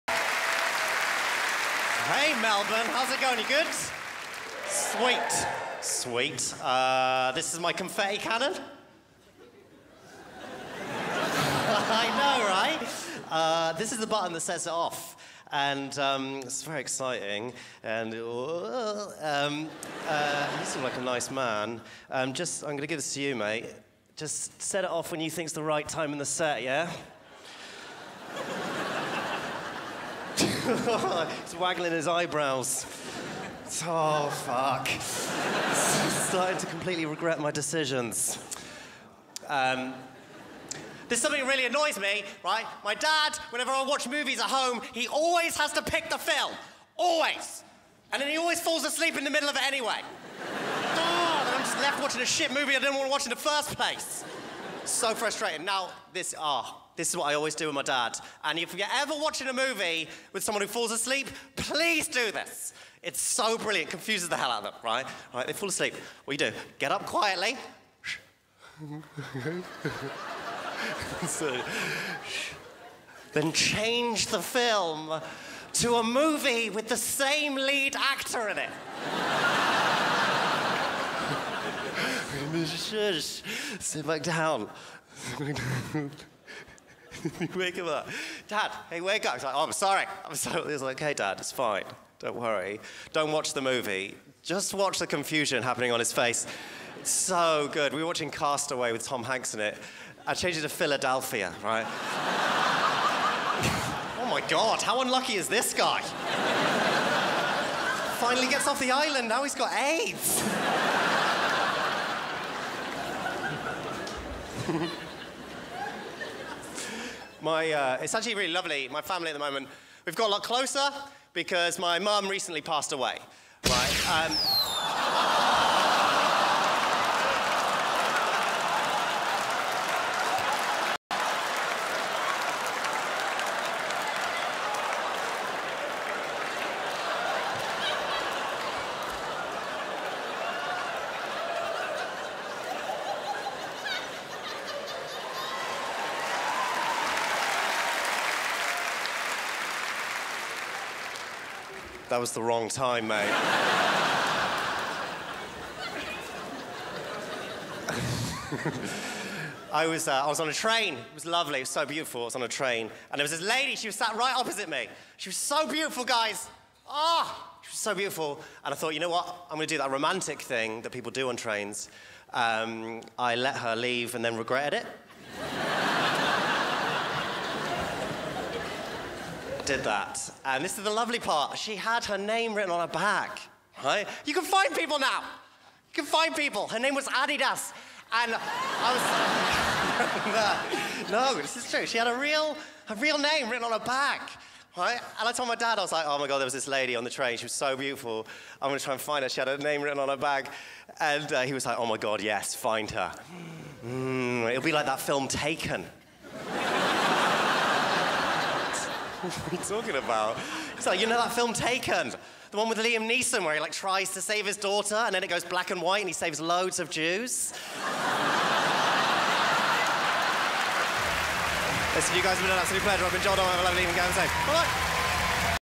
Joel Dommett - 2016 Melbourne International Comedy Festival Gala
JoelDommett brings a confetti canon to the stage, but wait til it goes off. Melbourne International Comedy Festival Gala for Oxfam 2016.

More laughs?